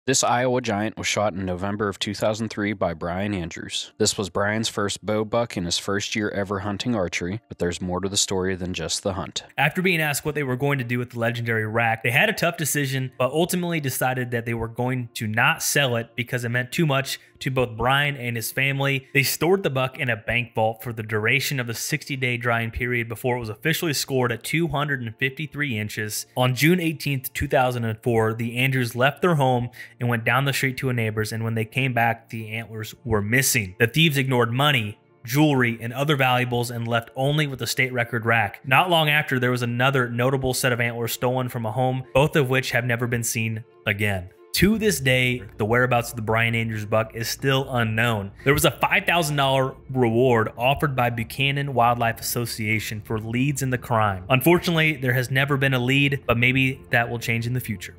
This State Record Was STOLEN Out Of His House!
This buck was shot in November of 2003 by Brian Andrews. This was Brian’s first bow buck in his first year ever hunting archery, but there’s more to the story than just the hunt.